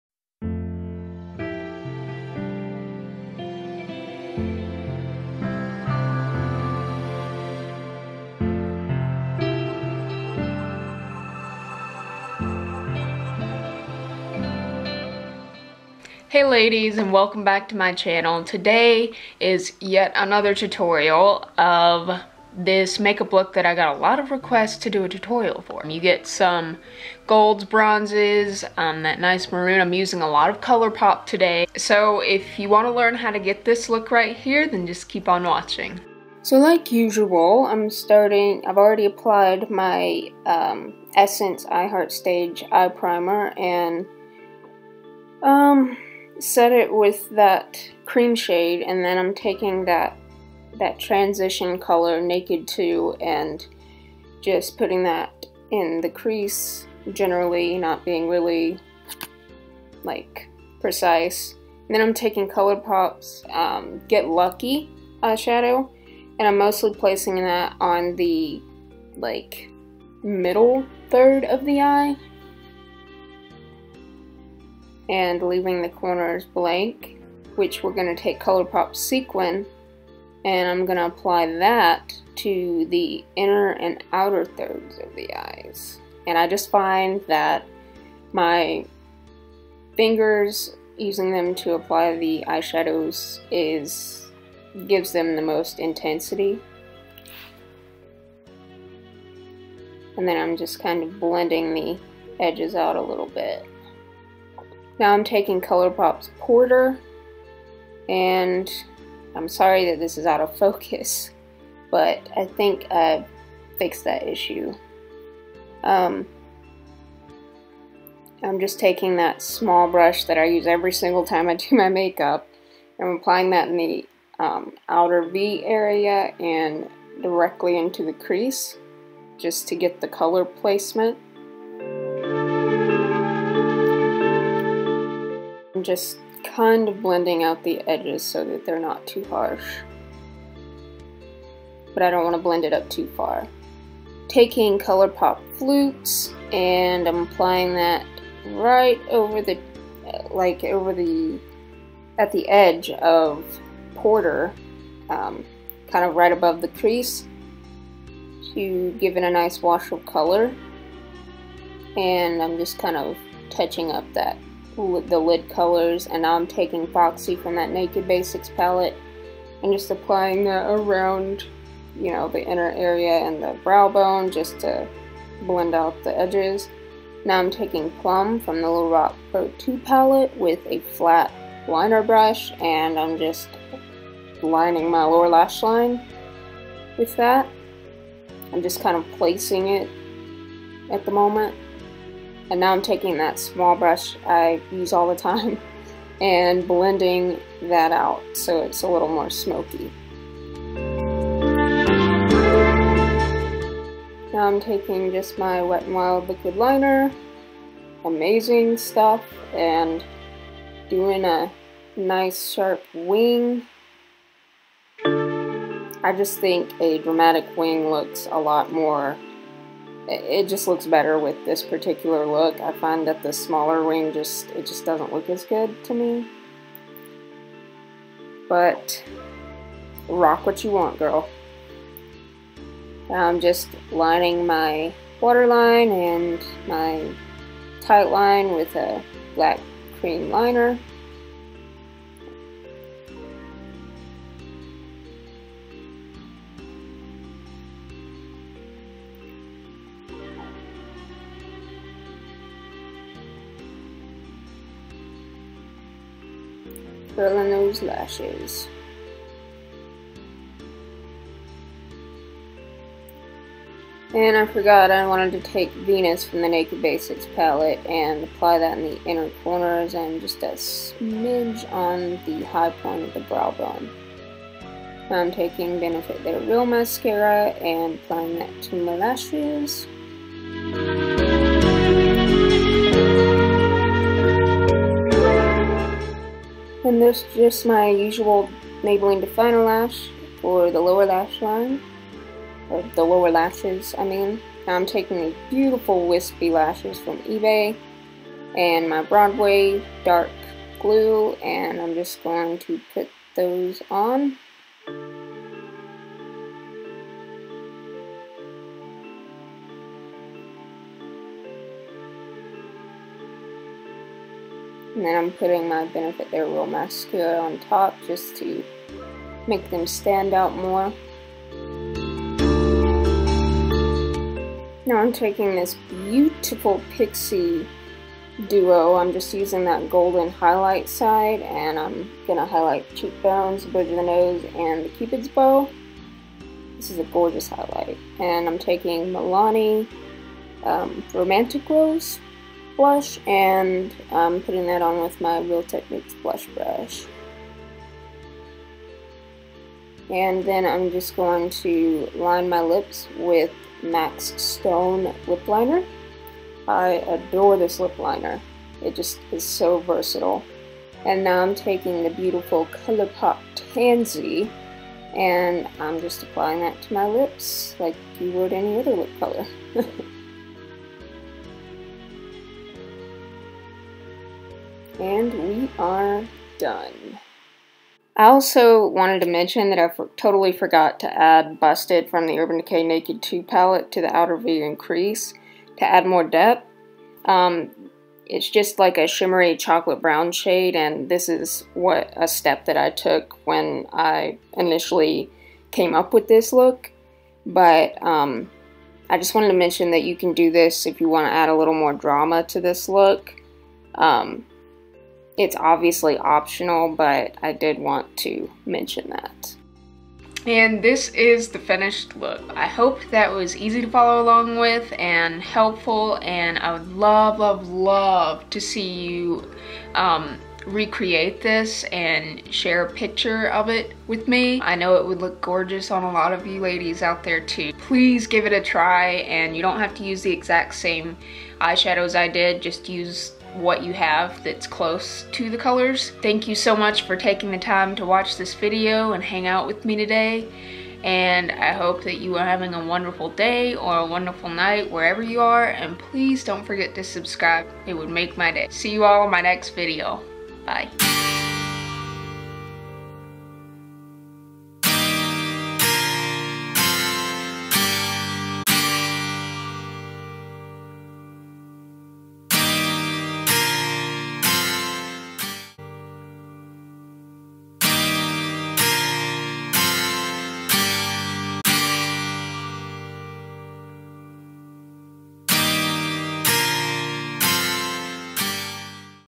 ♡ Golden Cabernet....Makeup Tutorial ♡
Products used:
Essence I Heart Stage Eye Primer
Urban Decay Naked Basics Palette
ColourPop Eyeshadows in:
Get Lucky
Sequin
Porter
Flutes
Lorac Pro 2 Palette
Wet n Wild Mega Liquid Liner in Black
Sally Hansen Lash Curler
Benefit They’re Real Mascara
Maybelline Define a Lash Mascara
eBay Super Wispy Lashes
Broadway Dark Lash Glue
Pixi Blush Duo in Rose Gold
Milani Matte Blush in Romantic Rose
Mac Lip Liner in Stone
ColourPop Liquid Lip Color in Tansy
Urban Decay Naked 2 Palette (Busted shade)